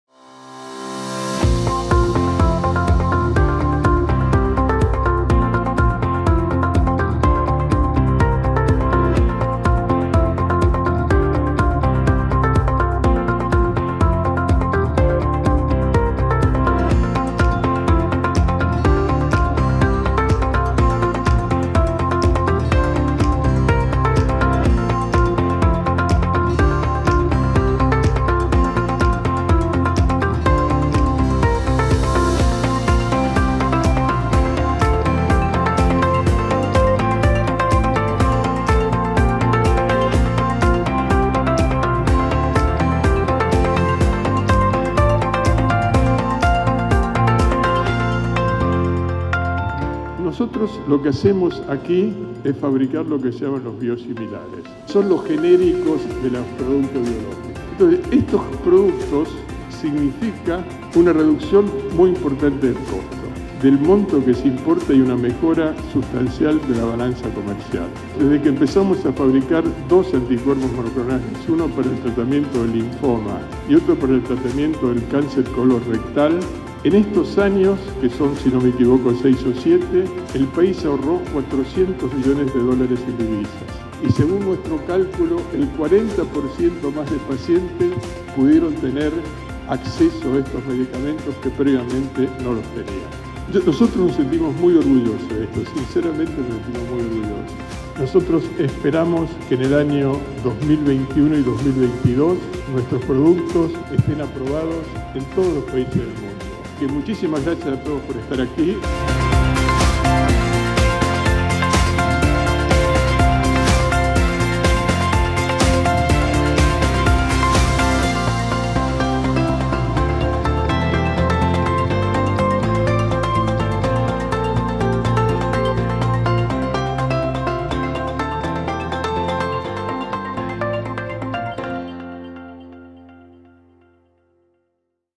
mAbxience | Inauguración de nuestra nueva planta en Garín.
"Fabricamos biosimilares, los genéricos de los productos biológicos.
Esto productos significan una reducción muy importante del costo, del monto que se importa y una mejora sustancial en la balanza comercial.

Desde que empezamos a fabricar dos anticuerpos monoclonales: uno para el tratamiento del linfoma y otro para el tratamiento del cáncer colorrectal, el país ahorró 400 millones de dólares y el 40% más de pacientes pudieron tener acceso a estos medicamentos que antes no tenían".

- Hugo Sigman